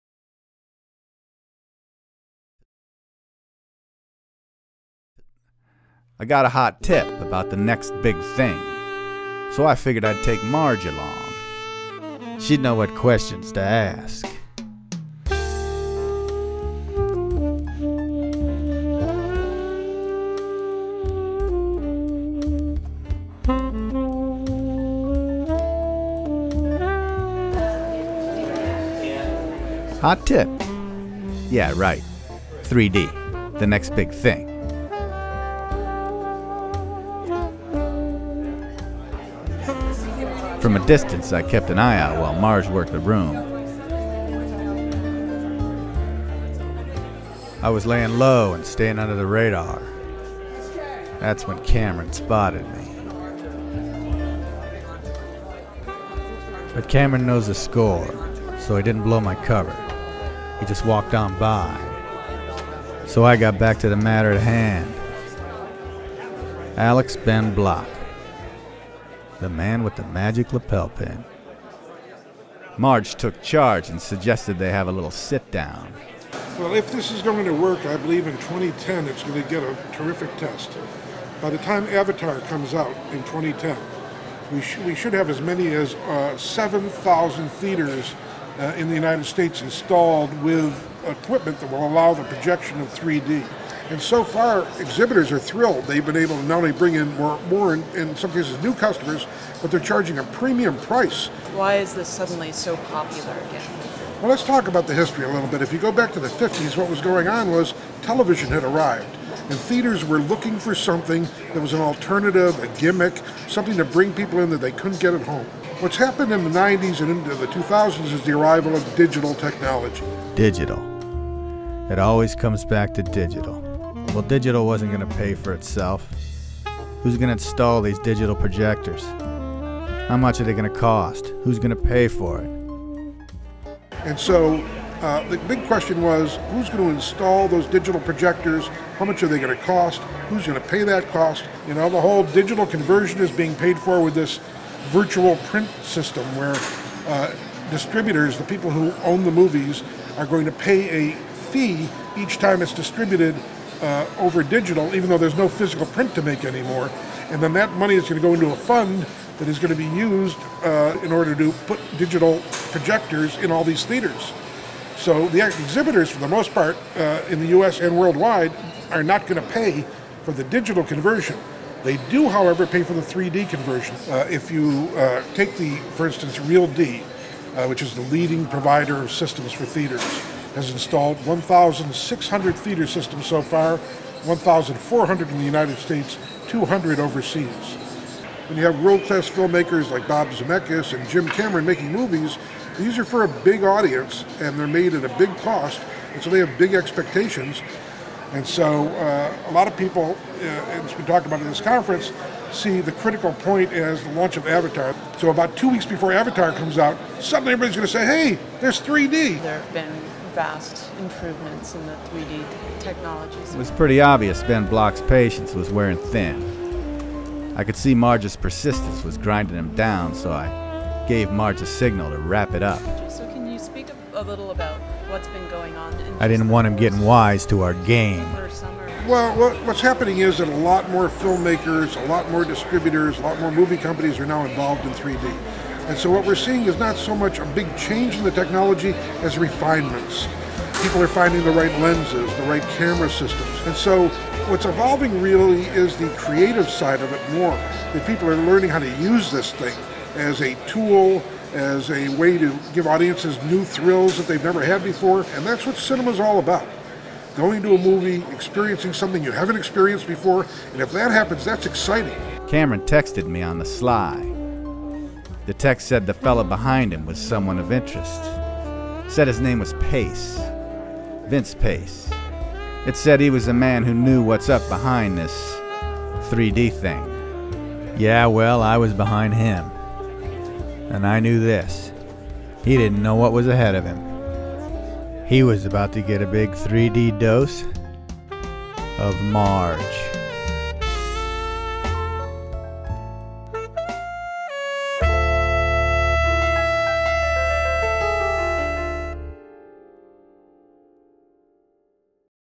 3D The Business - Tech Tips
3D first appeared in movie theaters back in the 1950s, when now-iconic photographs were taken of paper-glasses wearing theater-goers gaping at visual trickery.  Although the technology made random appearances throughout the next four decades, 3D was never fully embraced.  So why are people like film maker James Cameron now enthusiastically promoting 3D?   This months video primer explores the underbelly of this revitalized technology.